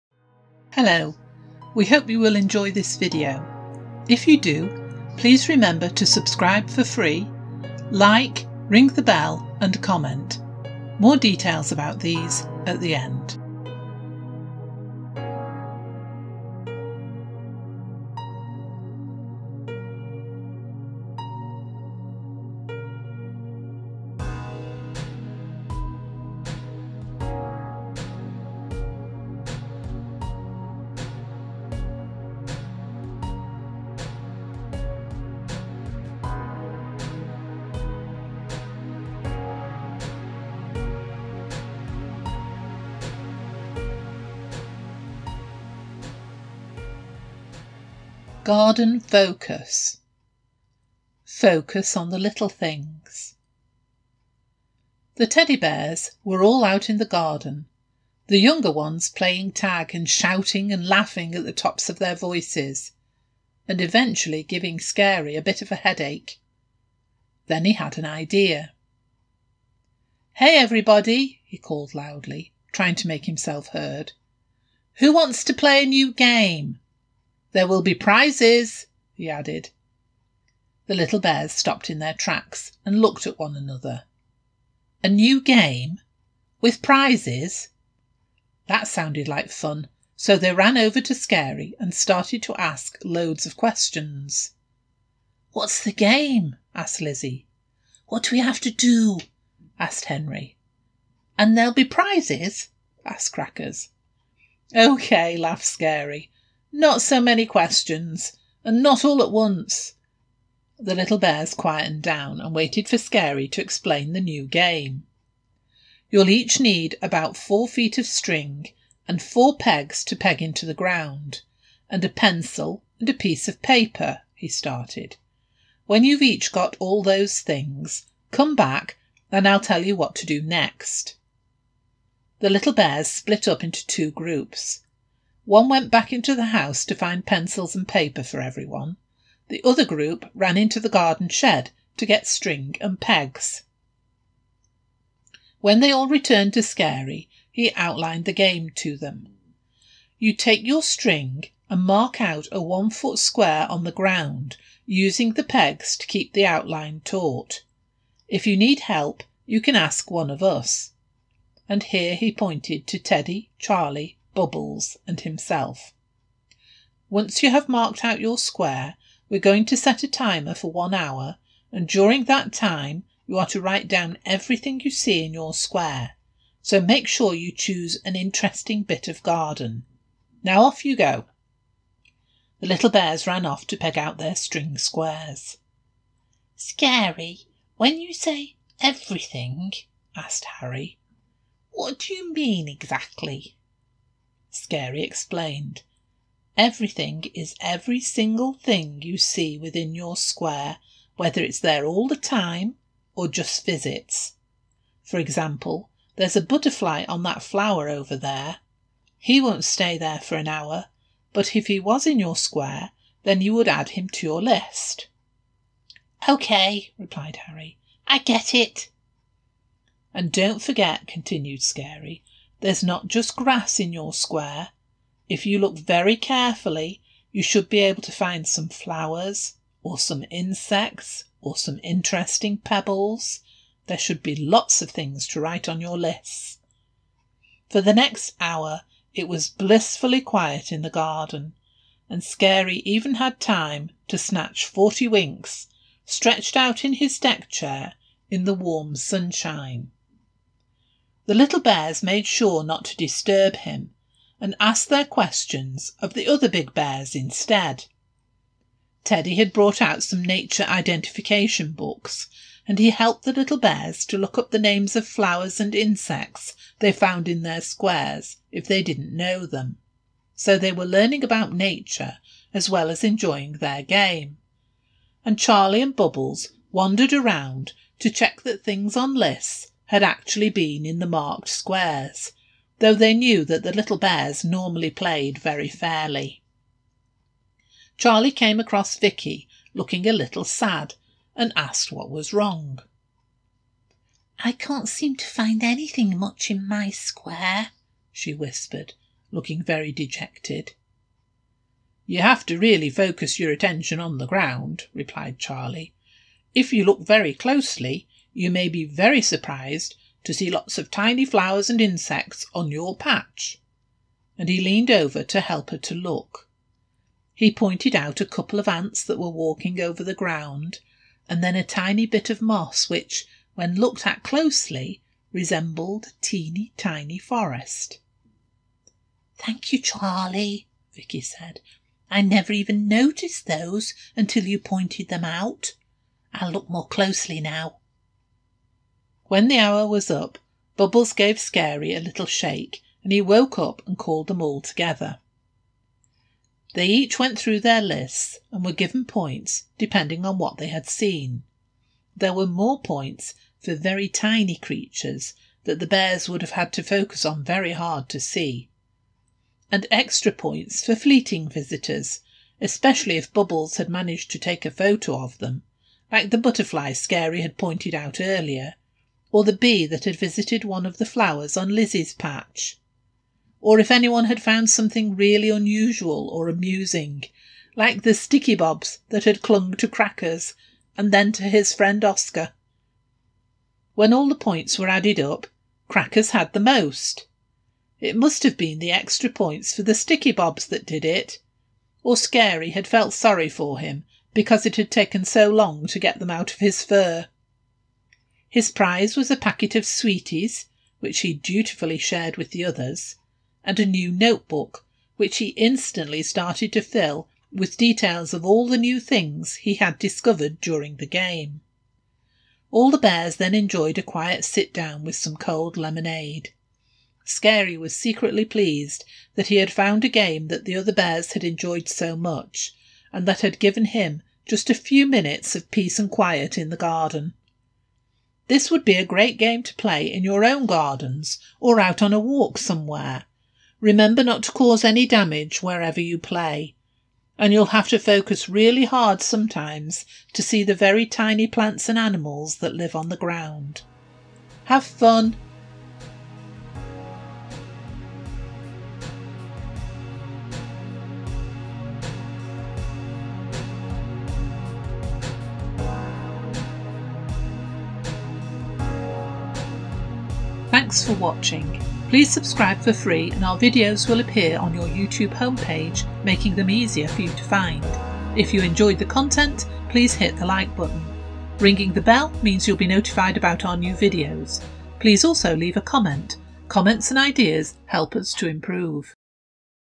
The Teddy Bears Play A New Game In The Garden
This is a Read-Along Story about an activity for children which they might like to try out and maybe also learn some interesting facts.  It is about learning a new game to play in the garden.  With our elite team of Teddy Bears as guides, it includes what equipment is required, how the game works, what there is to gain from the game.